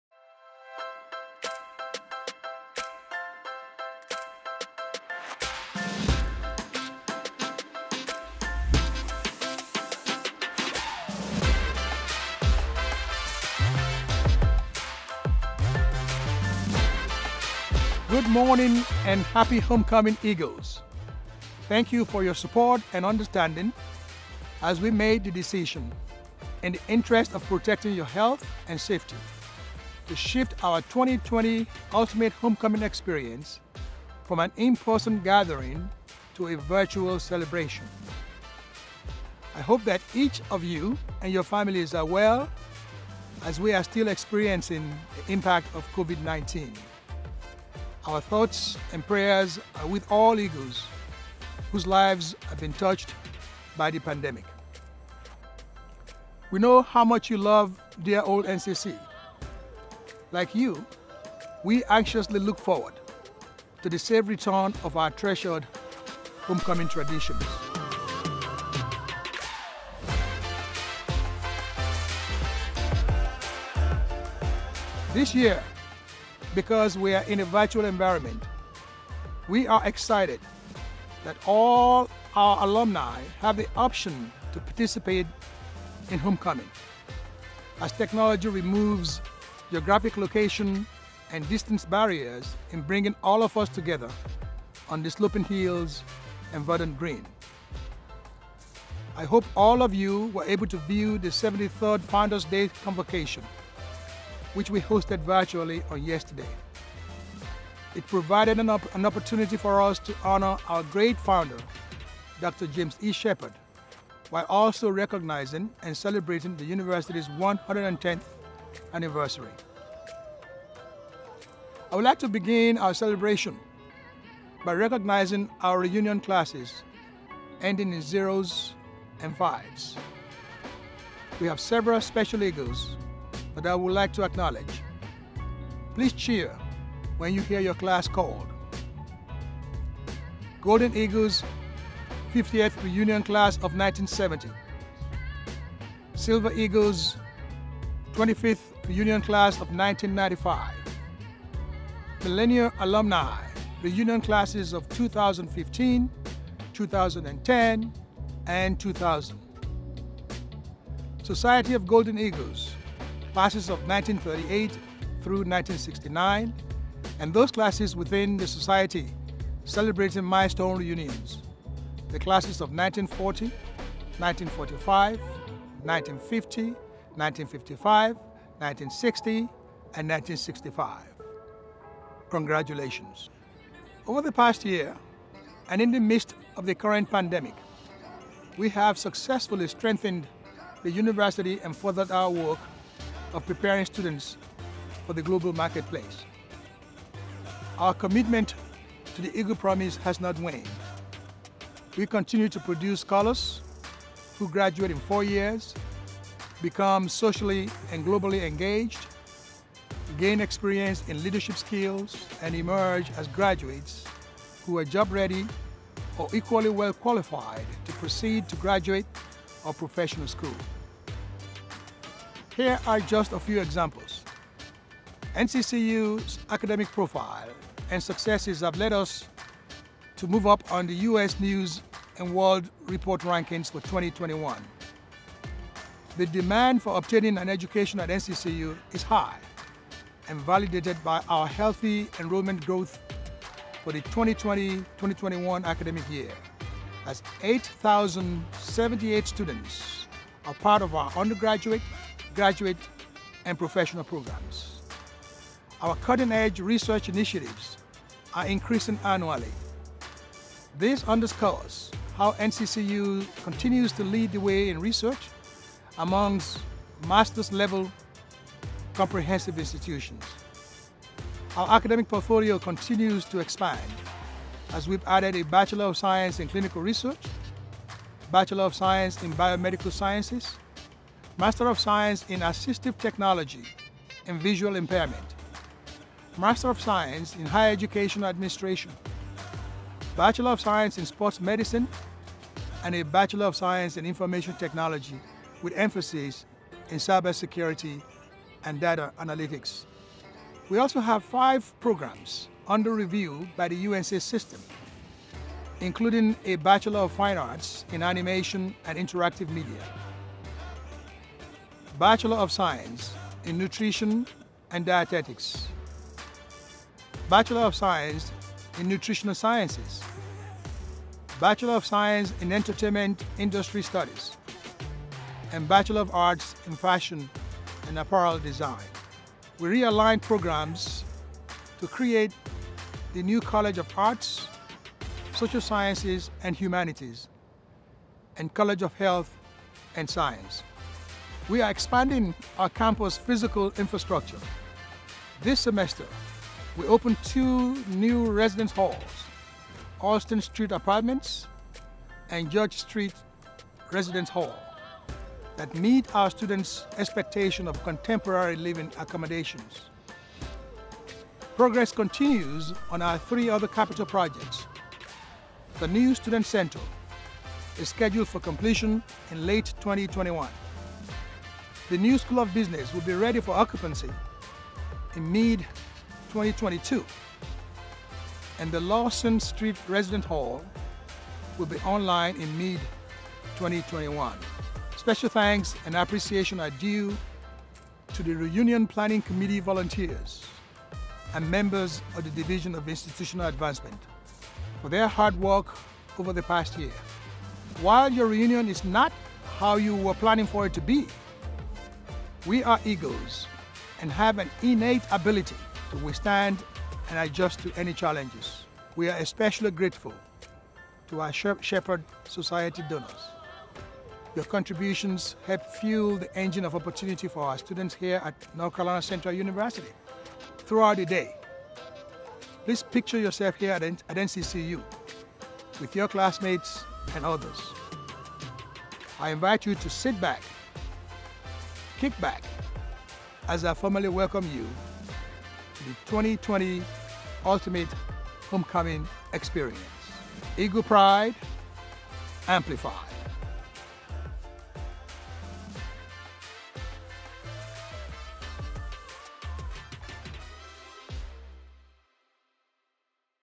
Chancellor Johnson O. Akinleye’s 2020 Ultimate Homecoming Experience Welcome Message
NCCU Chancellor Johnson O. Akinleye provides updates to alumni and friends for the 2020 Ultimate Virtual Homecoming Experience on Oct. 31, 2020.